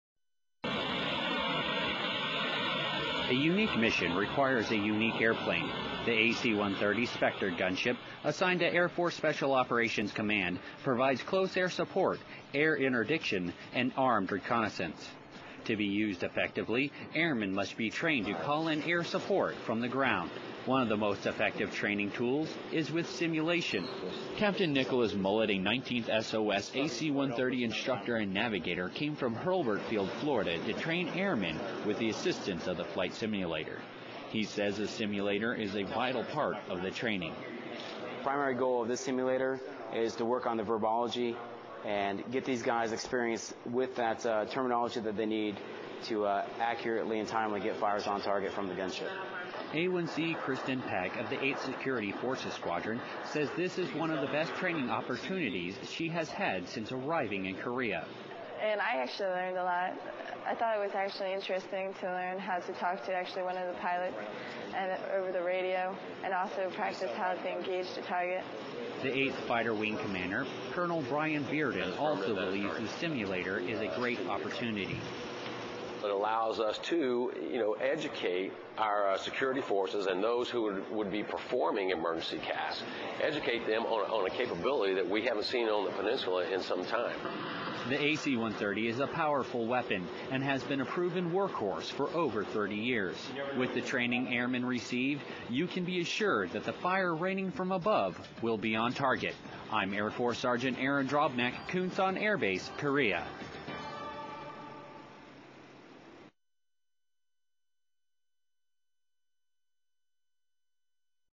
AC-130 Flight Simulator
ARMY.MIL - Air support is vital to combat missions. Support from the AC-130 Spectre Gunship is one of the ways Troops on the ground are able to send fire down to the enemy below.